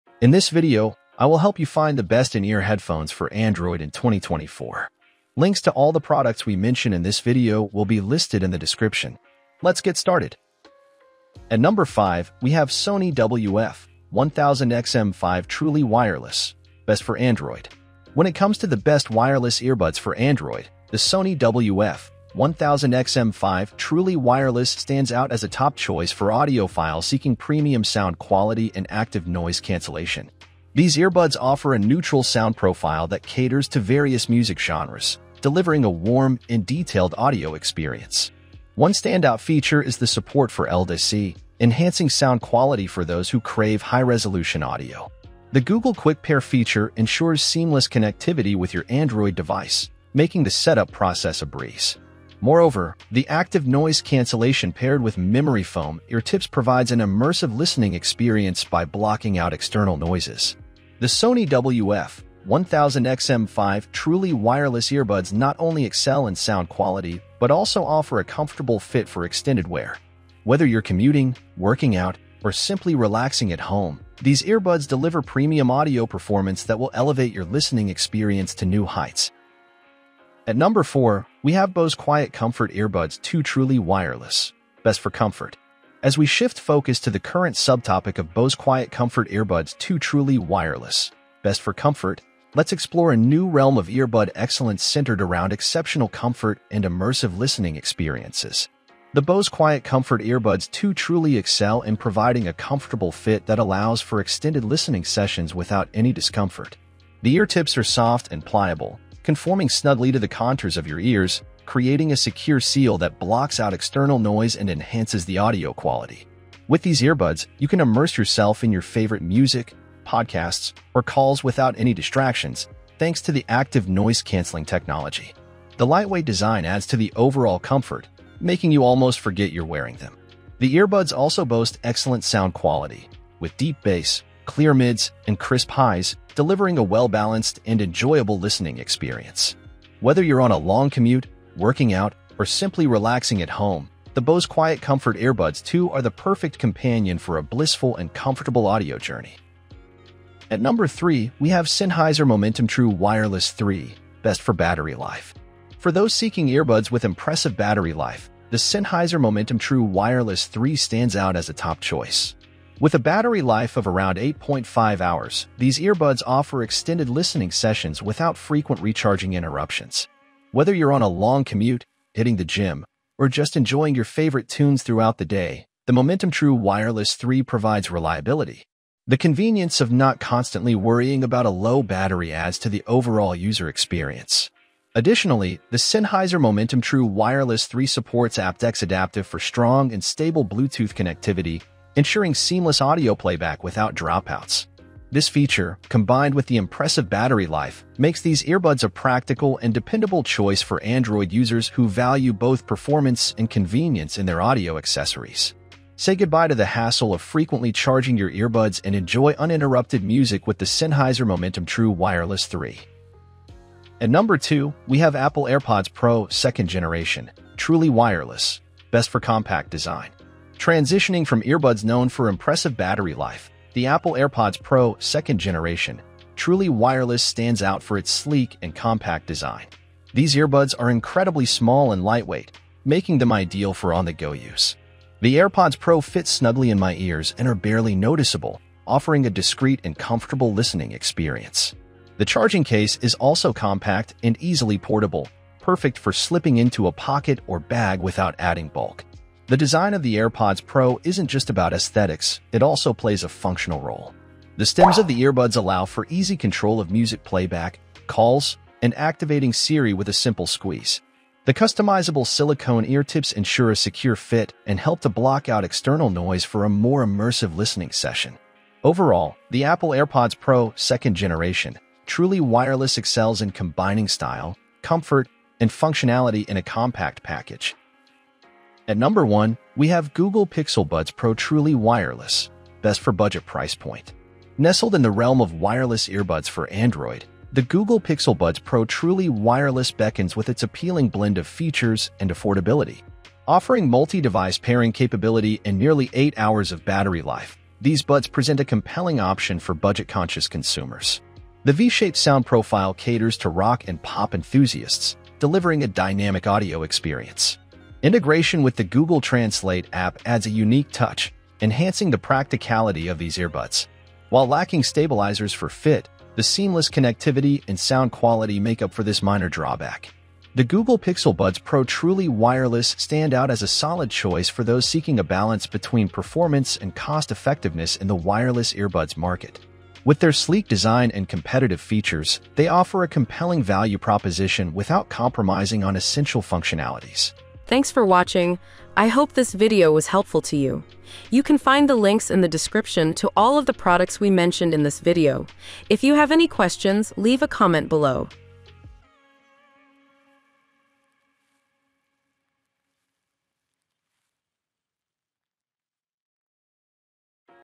Best In Ear Headphones For Android in 2024: Top Picks and Reviews
► Links to the best in ear headphones for android we listed in this video:

► Sony WF1000XM5 Truly Wireless
► Bose QuietComfort Earbuds II Truly Wireless
► Sennheiser MOMENTUM True Wireless 3
► Apple AirPods Pro 2nd generation Truly Wireless
► Google Pixel Buds Pro Truly Wireless

Today, we reviewed the 5 best in ear headphones for android you can buy in 2024. We prepared this list based on various tests, user reviews, and real life experiences. We tried to prepare a list that is suitable for budget conscious buyers that are looking for affordability and reliability at the same time. At the same time, we included some premium picks for those looking to get a high-end experience.